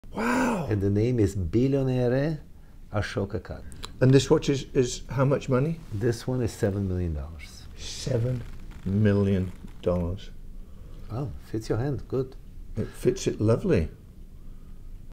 7 million dollar watch!
On my latest visit to Jacob & Co, I got my hands on the $7m Billionaire Ashoka watch. It's a masterpiece of jewelry and horology - I think I'm in love!